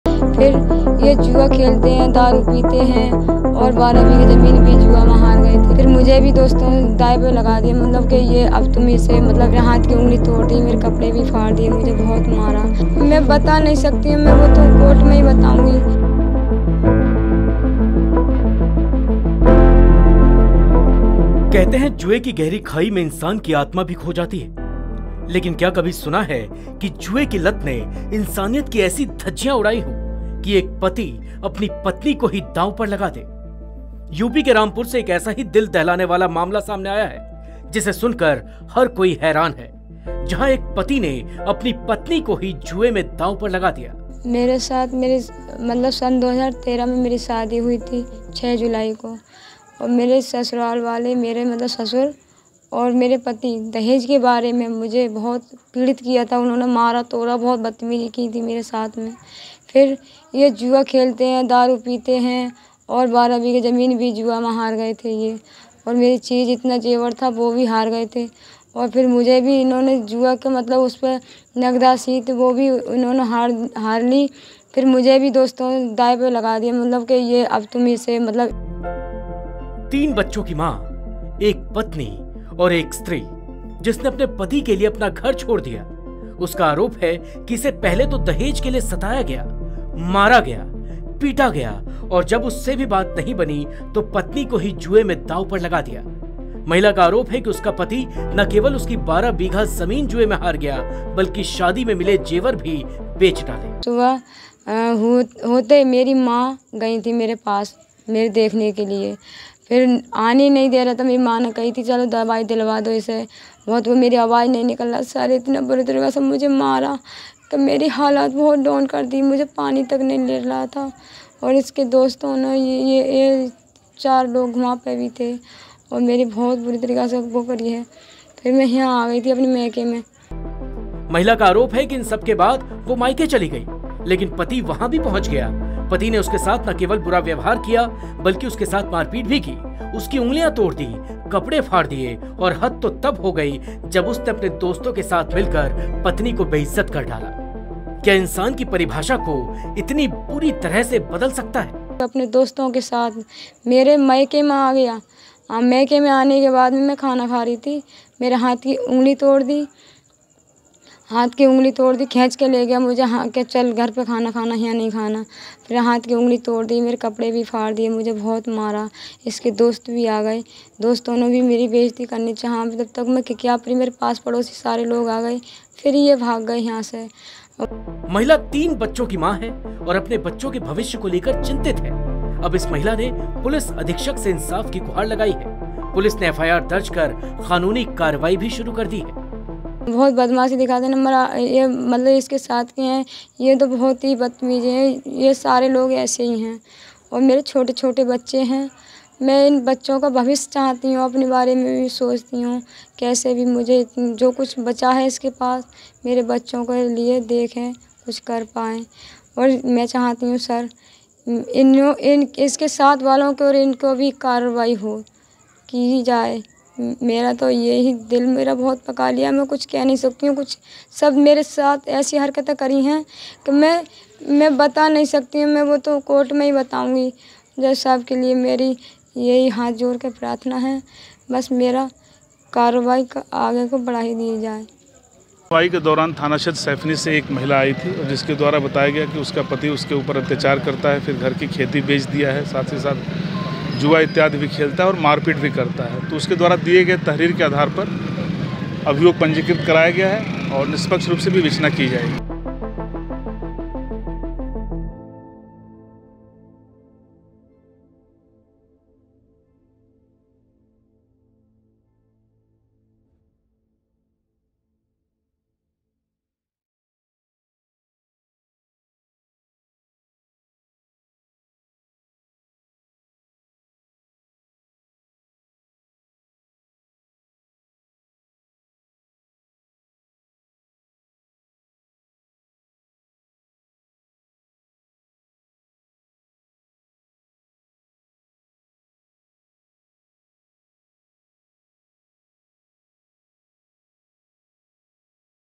Gambling Addiction: Rampur में जुए में हार के बाद पति ने पत्नी को दांव पर लगाया | Uttar Pradesh News
Rampur News: कहते हैं. जुए की गहरी खाई में इंसान की आत्मा भी खो जाती है. लेकिन क्या कभी सुना है कि जुए की लत ने इंसानियत की ऐसी धज्जियां उड़ाईं हों कि एक पति अपनी पत्नी को ही दांव पर लगा दे. यूपी (Uttar Pradesh) के रामपुर (Rampur) से एक ऐसा ही दिल दहलाने वाला मामला सामने आया है. जिसे सुनकर हर कोई हैरान है. जहां एक पति ने अपनी पत्नी को ही जुए में दांव पर लगा दिया.